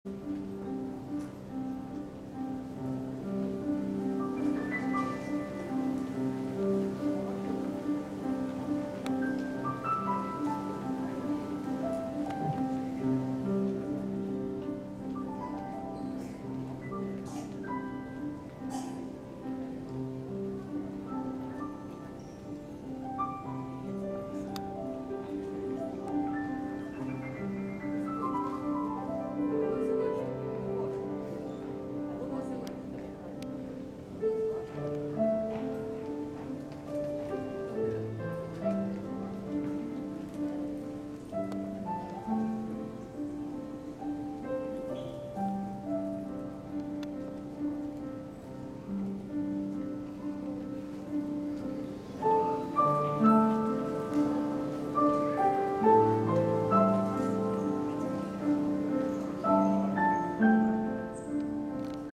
ロビーでピアノ生演奏が披露される帝京大学溝口病院
帝京大学医学部附属溝口病院では、患者さんやそのご家族などが待合い・休憩しているロビーで、ピアノの生演奏が披露されています。